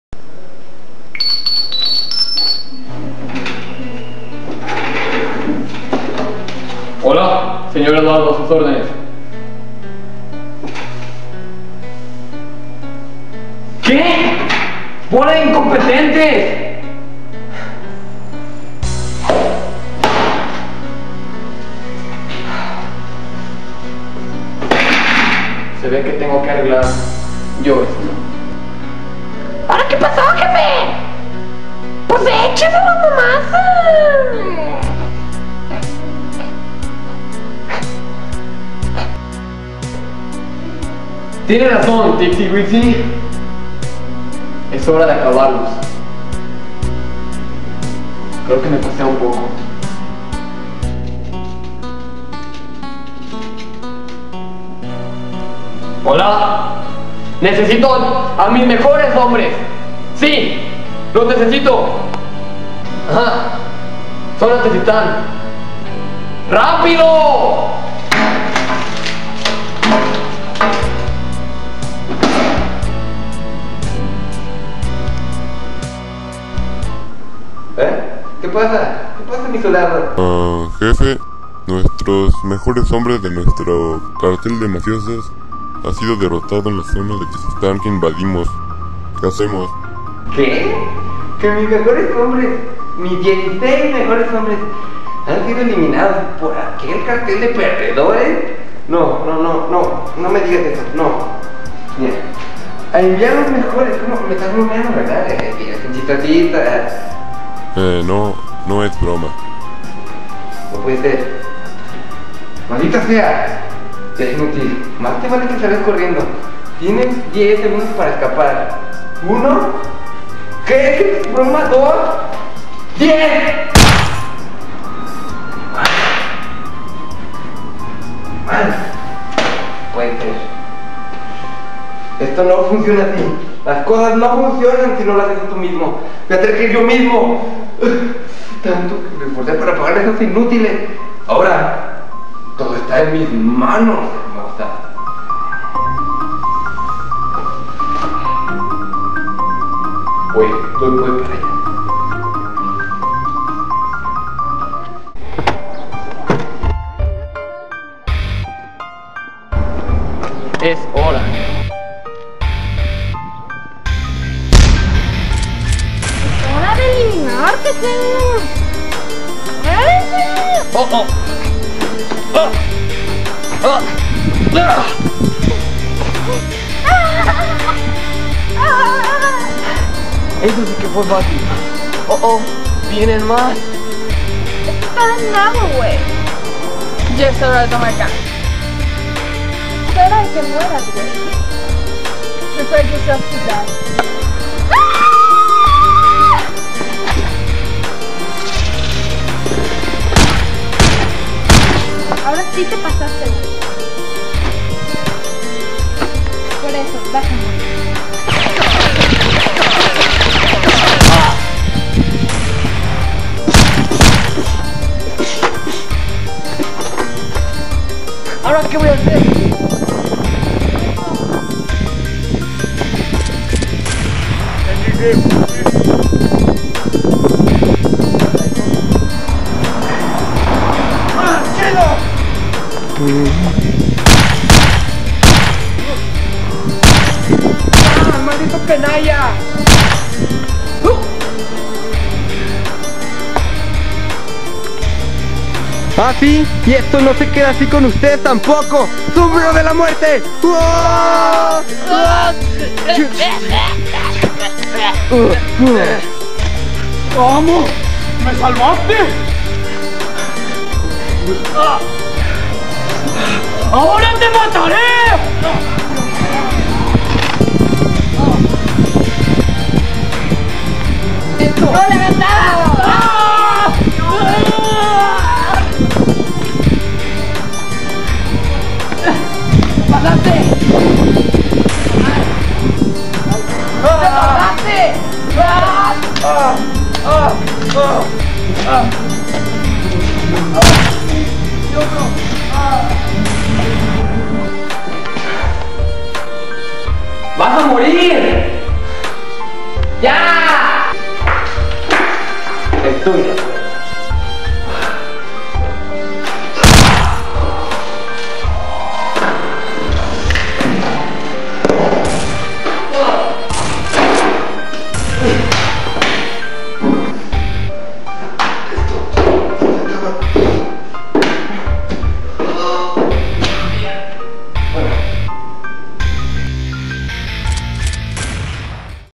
Mafiosos
Un Cartel de Mafiosos "dueños" de la zona de Tesistan es saboteado por oto cartel Enemigo que tiene el objetivo de adueñarse de ese territorio para hacer sus negocios sucios.
¿Quien se quedara con la zona de Tesistan?
AVERIGUENLO.

Con aparciones de:
Eduardo Gonzalez (Jefe Mafioso 1)
Fernando Valdes (Jefe Mafioso 2 y asesino de tejado)
Carla Gonzalez (Extra y asesina)
Fernanda (Extra y asesina)
Antonio (Extra y Refuerzo de Mafioso 1)